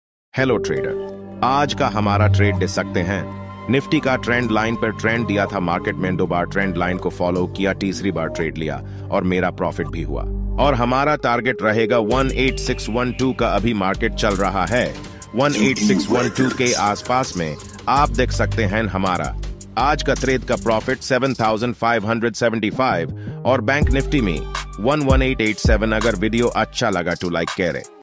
Trend line pe trad kaise kare aur Aaj kaa profit 😨 @tradingwallah7
Trend line pe trad kaise kare aur Aaj kaa profit 😨

Hello trader
Aaj ka hamara trade de sakte hain Nifty kaa
Trend line per Maine trend diya tha
Market mein do bar trend line ko follow Kiya
Aa Teesri bar Maine trade liya aur mera profit bhi hua
Aur hamara target rahega 18612 ka
Abhi market chal raha hai 18612 ke aas paas me
Aap dekh sakte hain hamara aaj ka trad ka profit 7575 aur banknifty me 11887
Agar video achcha Laga to like Kare